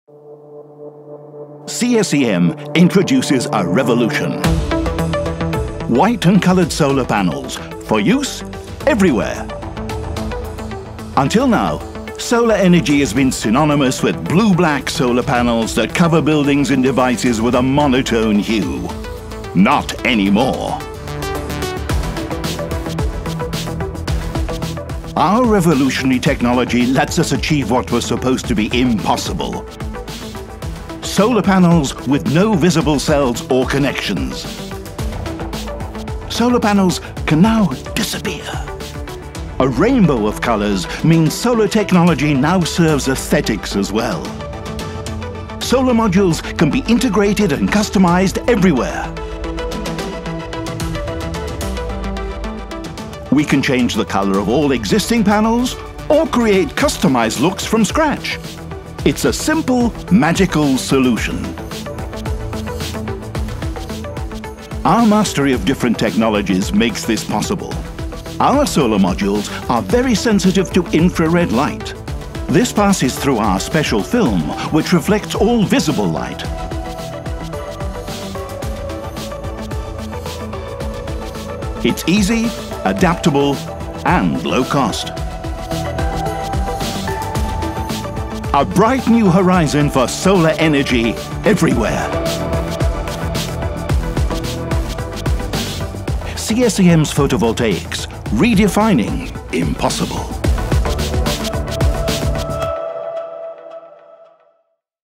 White and colored solar modules: a revolution for building integration (BIPV)
Until now, solar energy has been associated with blue-black photovoltaic panels that cover buildings and devices with a monotone hue. The appearance is visually unappealing and this has hindered the acceptance of photovoltaics by built-environment professionals. Not anymore…
For decades, architects have been asking for a way to customize the color of solar elements to make them blend into a building’s skin. White is a particularly interesting color as it is widely used for its elegance, versatility, and fresh look. Despite this demand, no one has been able to realize a truly white solar module. In fact, most believed that it was inconceivable because most of the light is reflected, precisely contrary to solar panel requirements.
Our revolutionary technology lets us achieve what was supposed to be impossible: white and colored solar panels with no visible cells or connections. It can be applied on top of an existing module or integrated into a new module during assembly, on flat or curved surfaces. We can change the color of all existing panels or create customized looks from scratch. Solar panels can now disappear; they become virtually hidden energy sources.
It’s an easy, adaptable and low-cost solution.